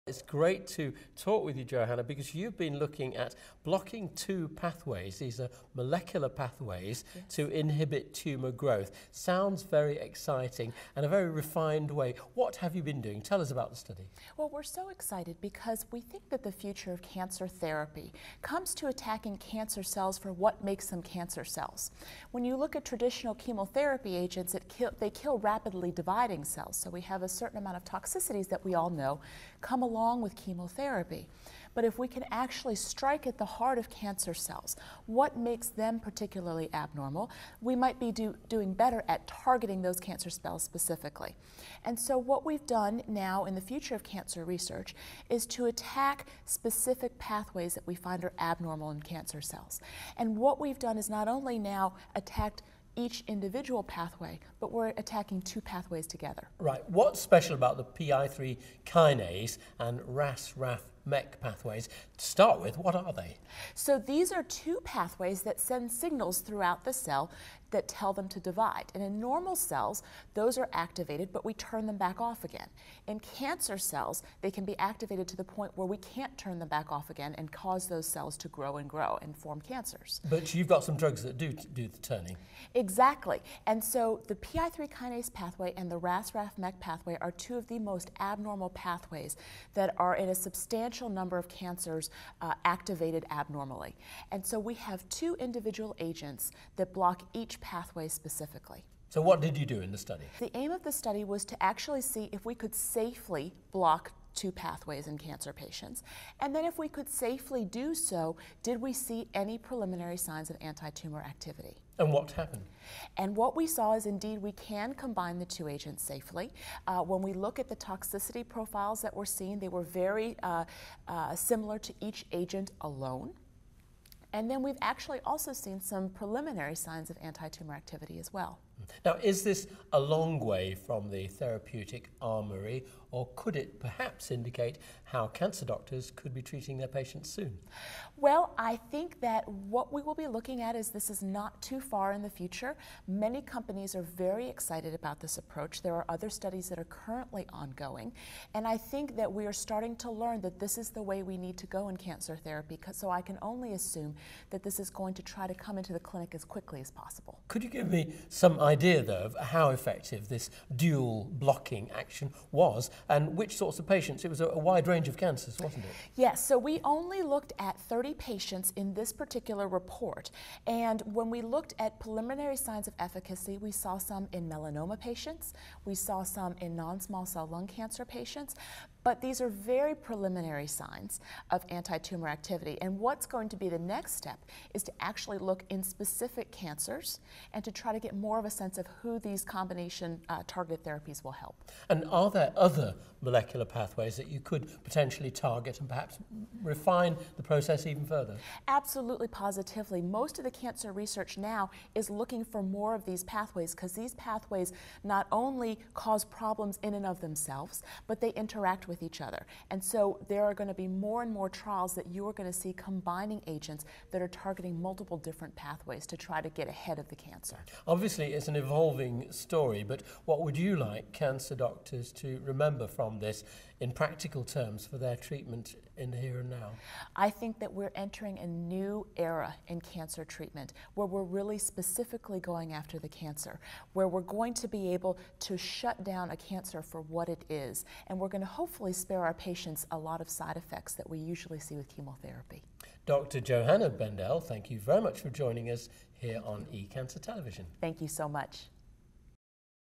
AACR 2011: Combination therapy targeting MEK and PI3K pathways shows anti-tumour activity
Dr Johanna Bendell - Sarah Cannon Research Institute, Nashville, USA.

The combination of two compounds that inhibit two of the most frequently mutated cancer pathways is showing promise in an ongoing Phase I trial. The research, presented by Dr Johanna Bendell, tests a combination of GDC-0973, which inhibits MEK1/2 and GDC-0941, which inhibits PI3K. The patients received a combination of different doses of GDC-0973 and GDC-0941; these drug combinations were well tolerated and only produced with mild side effects. Several patients demonstrated decreases in tumour size, including two patients with melanoma, one with prostate cancer and two with non-small cell lung cancer. It is hoped that these targeted treatments will eventually help clinicians treat cancers more effectively and will produce fewer adverse effects than current therapies.